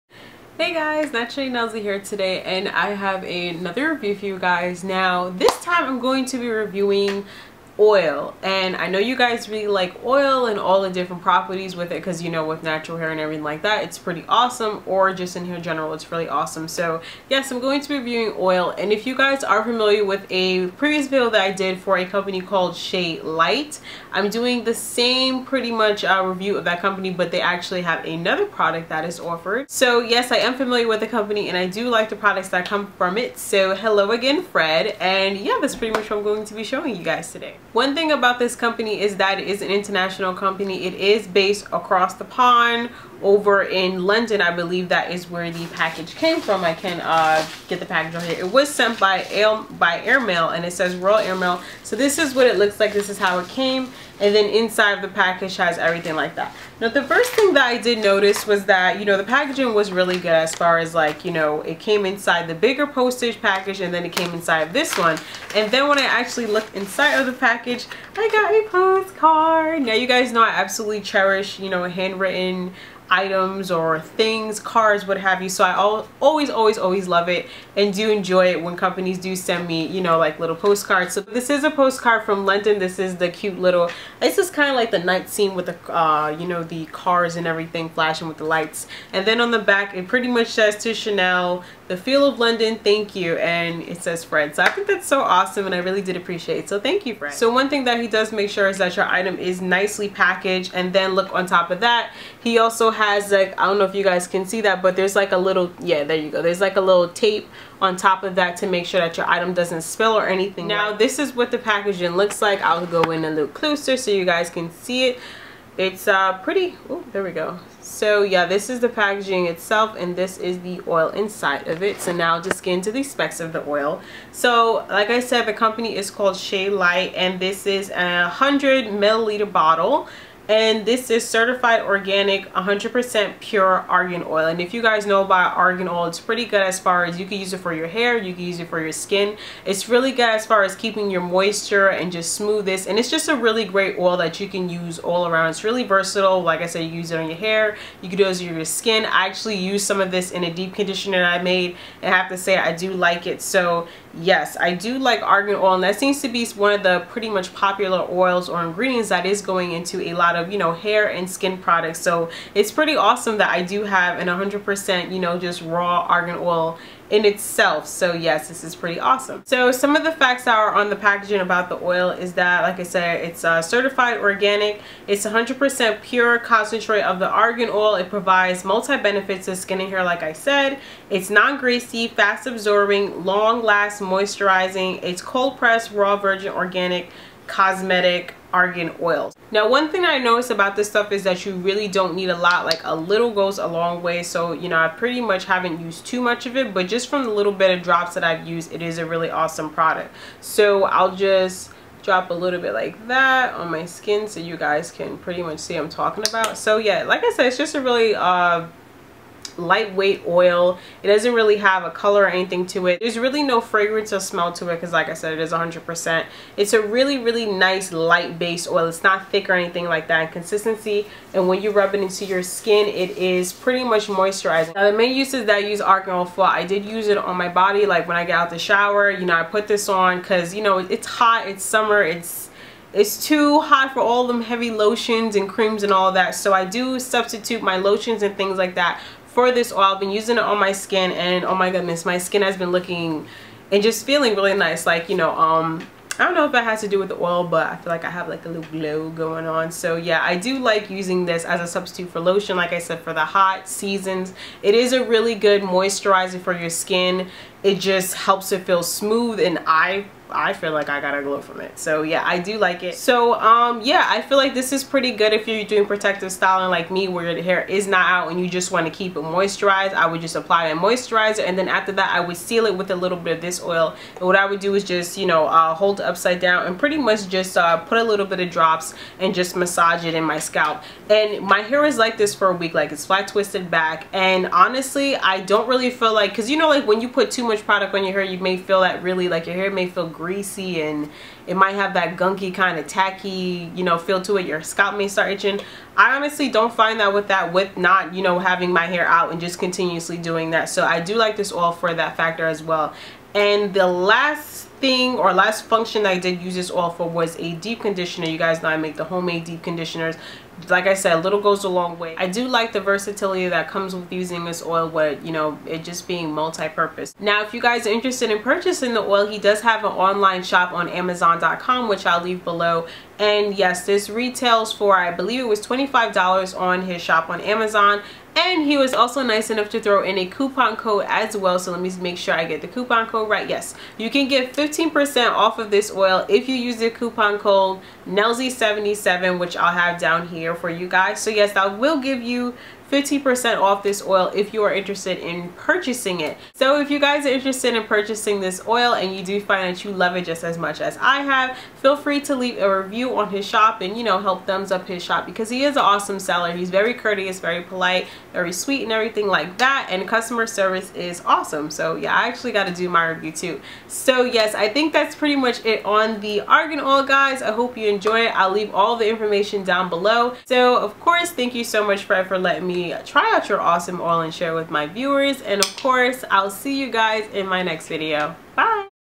Review | Shealite Argan Oil (Great for skin & hair)
Hey guys, back with another video showing you all some awesome argan oil from Shealite.
Argan Oil has many different benefits for healthy skin and hair and has a lot of versatility and can be used in many different ways!

FTC: The product shown in this video were supplied by Shealite. These are my honest and truthful thoughts, opinions and feedback.

FAQ
Hair Color: Golden Copper by Creme of Nature
Hair Style:
Camera: Canon Rebel T3i